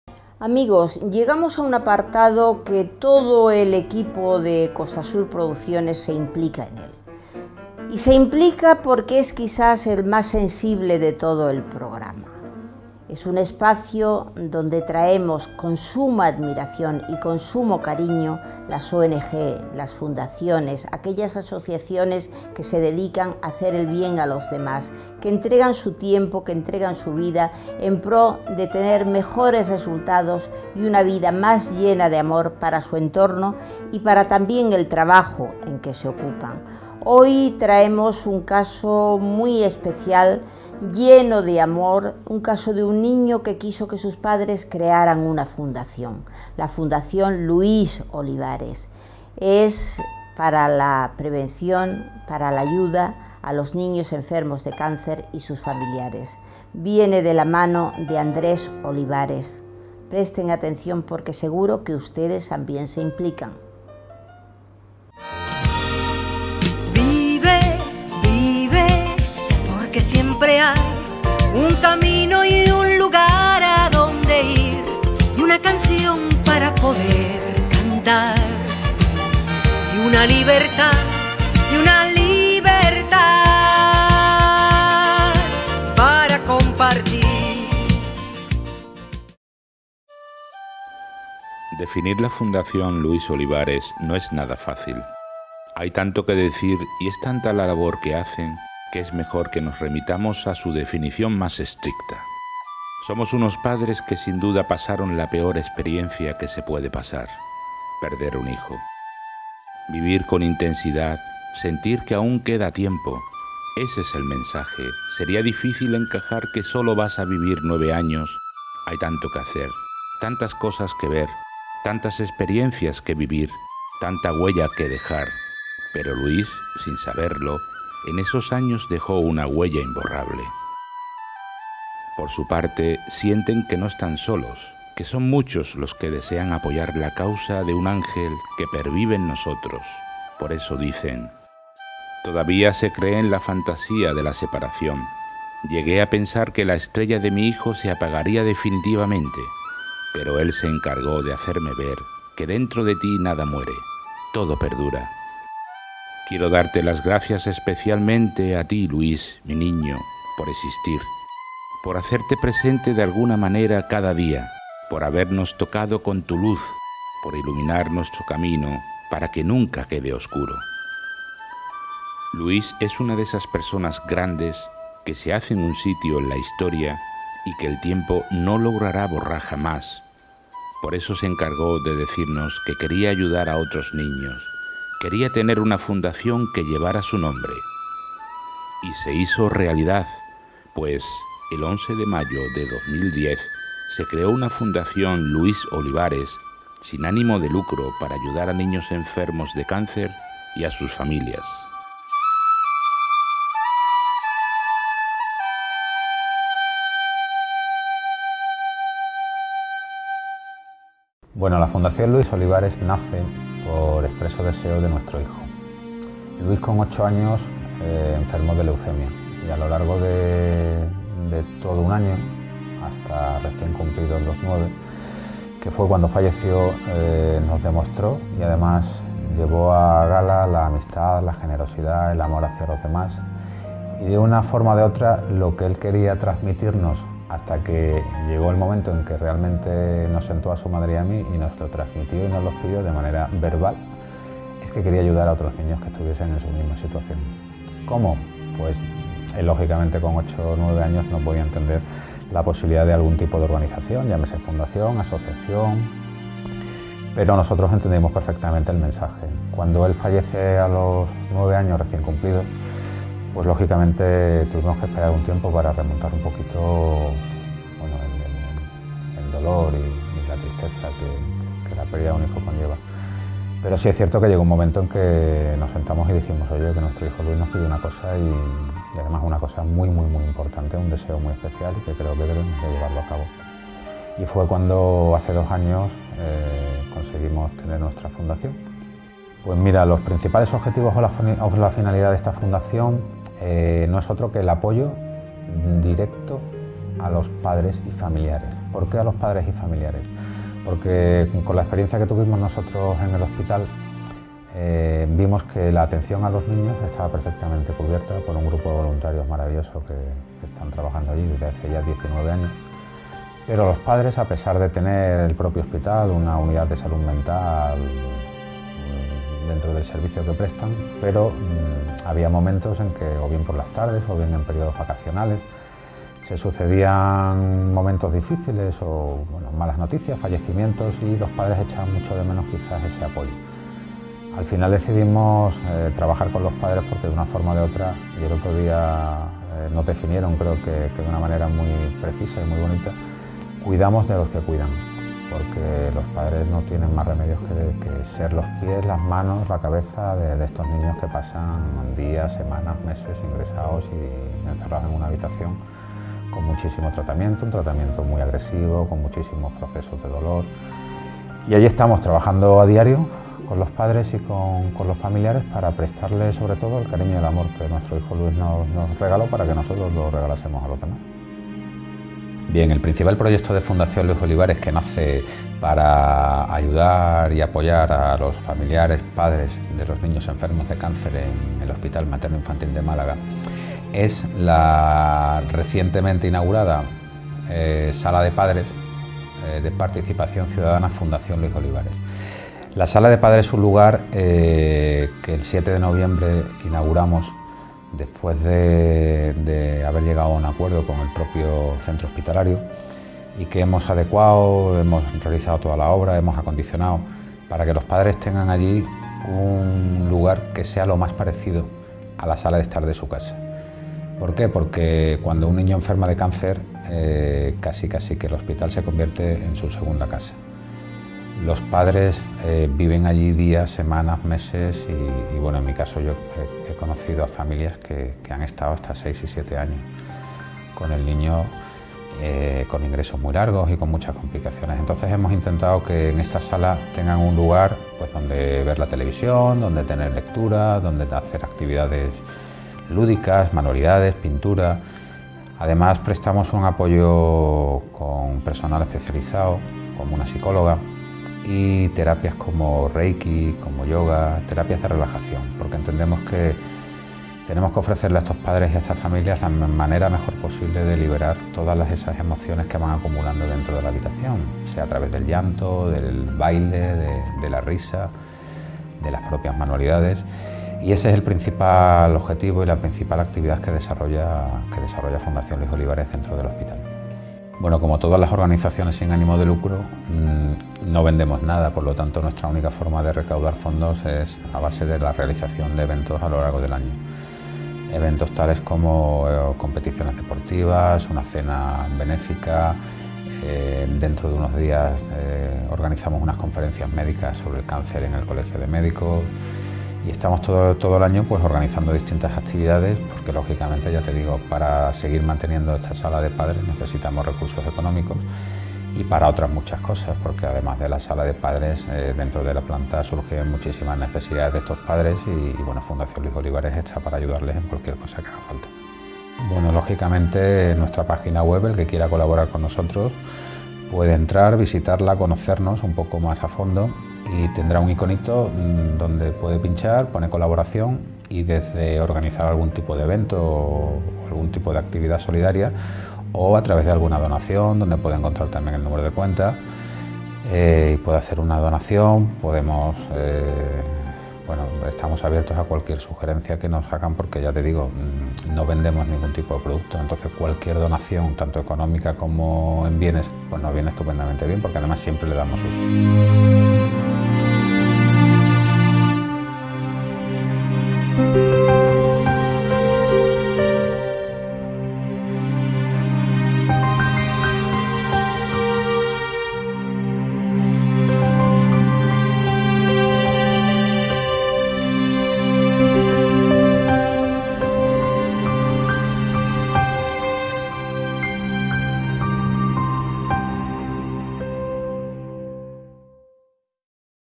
Fundacion Luis Olivares - SCV 1
En nuestro espacio UNIDOS SOMOS MÁS,  recogemos opiniones y desarrollo de asociaciones que dedican su tiempo en pro de la prevencion, ayuda y atención de los demás. Hoy con LA FUNDACION LUIS OLIVARES, dedicada a la atención y prevención del cáncer infantil